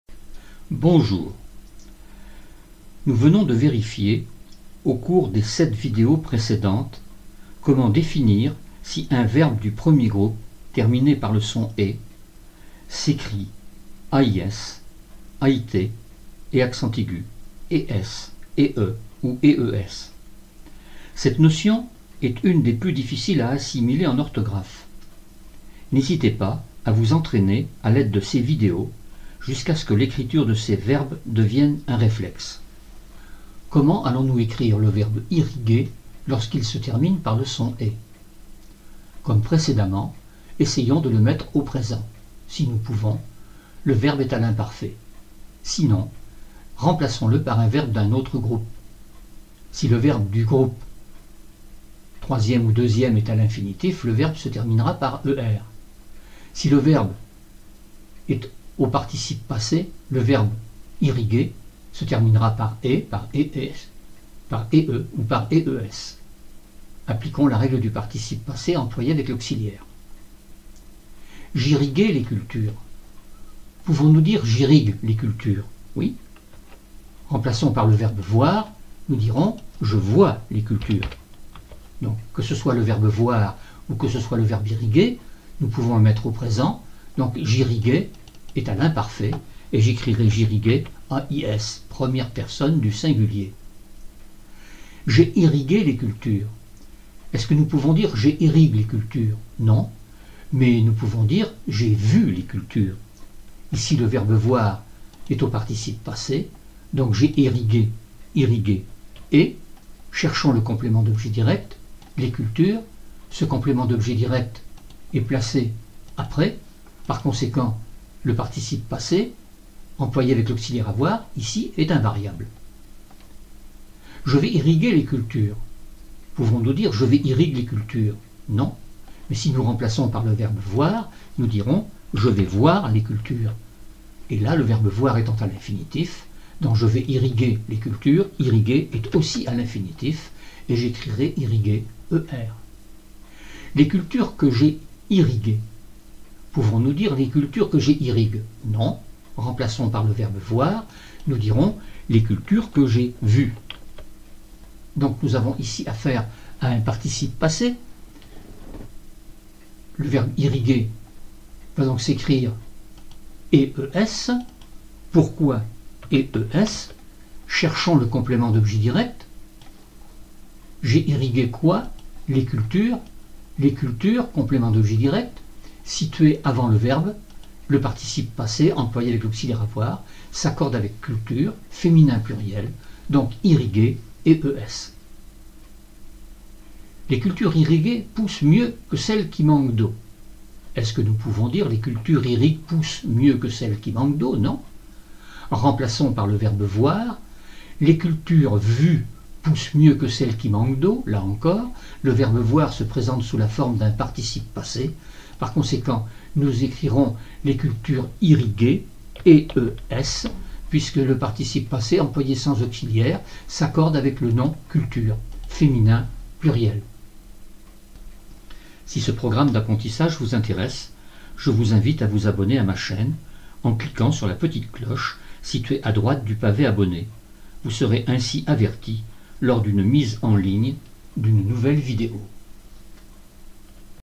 085-Les verbes du premier groupe terminés par le son é Révision8
Pour accéder aux autres vidéos de la chaîne vous pouvez cliquer sur le lien suivant :

Une difficulté majeure en orthographe est l’écriture des verbes du premier groupe lorsqu’ils se terminent par le son é.

Cette difficulté a été abordée dans les vidéos N° 5 à N° 9. J’ai pensé qu’il était souhaitable de revenir sur ces notions afin de bien s’habituer à faire la différence entre le verbe à l’infinitif, le verbe conjugué à l’imparfait et le verbe employé comme participe passé, soit seul, soit avec l’auxiliaire être ou avec l’auxiliaire avoir.

Dans les vidéos N° 78 à N°86 nous pouvons nous familiariser avec ces différentes situations en les présentant à l’aide d’un même verbe. N’hésitez pas à les regarder autant de fois qu’il sera nécessaire et à les mettre en pratique jusqu’à ce que la démarche proposée pour distinguer les différentes situations devienne un réflexe. C’est à ce prix que vous parviendrez à écrire de façon sûre la terminaison de ces verbes.